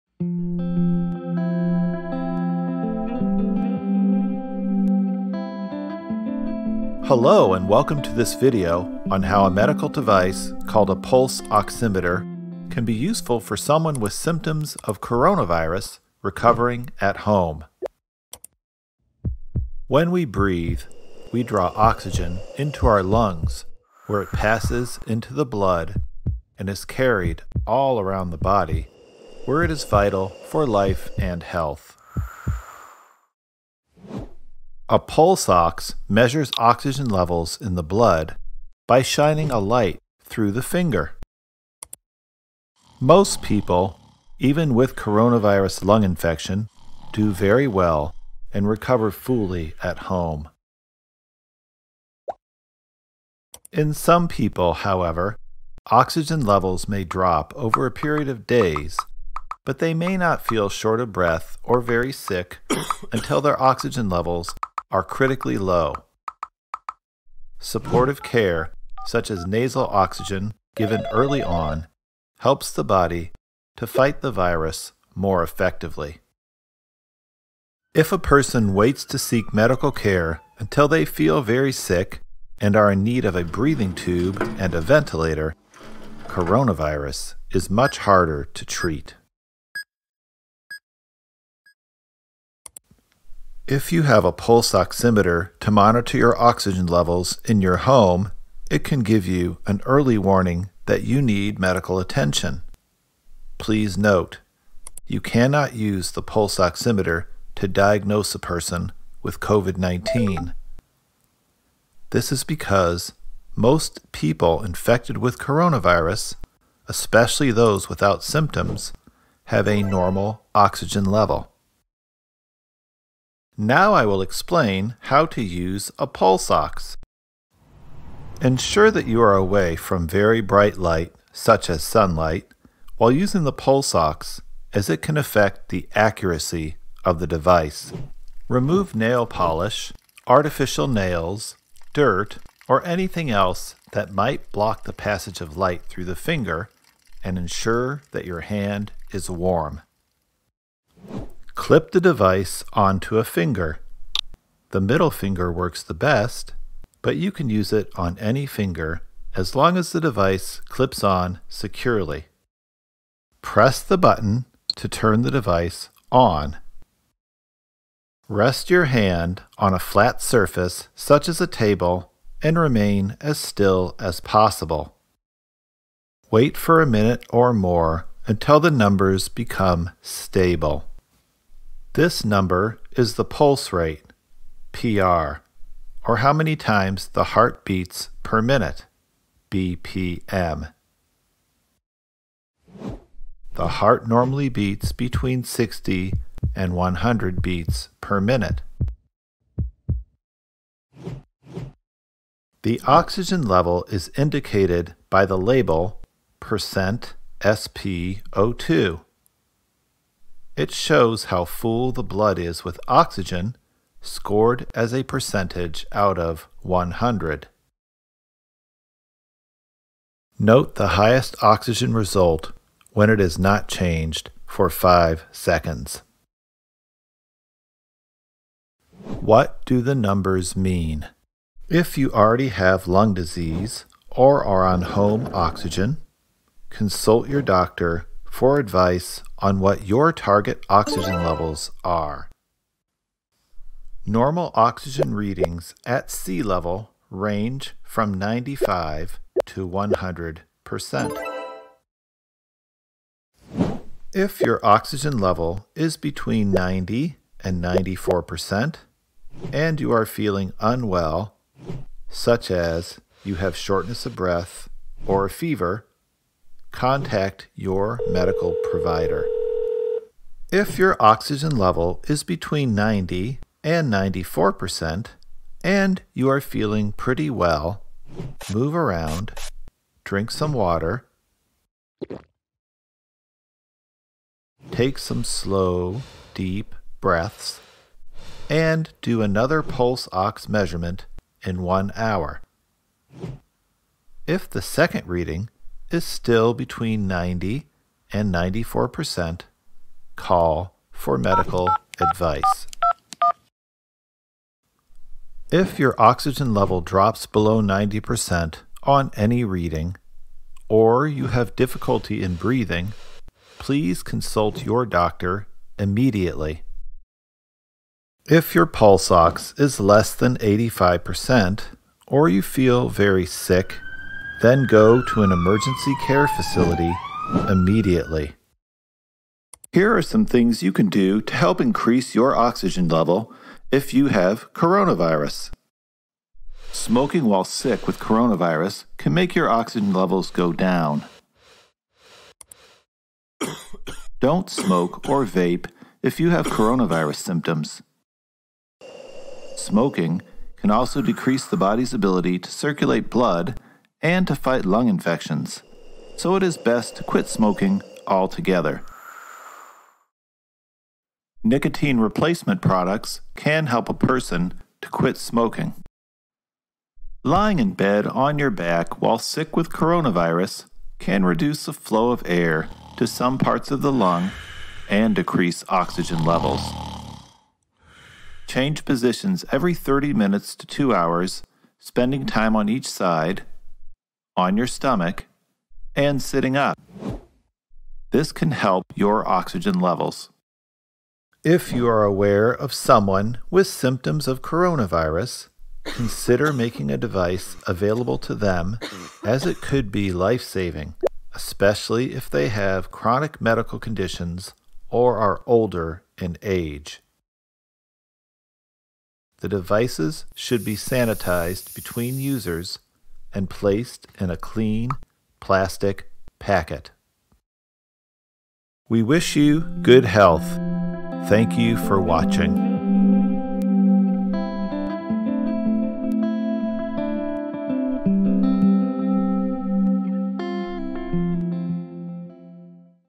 Pulse Ox in COVID-19 - How to Use
This video shows how a pulse ox device can be useful for someone with symptoms of coronavirus.   This animation was made by American Board Certified Physicians in Family Medicine and Lifestyle Medicine, Drs. Rick and Anita Gutierrez.  They have served in Durban, South Africa since 2002. This video is also available in Zulu Spanish French Portuguese and Hungarian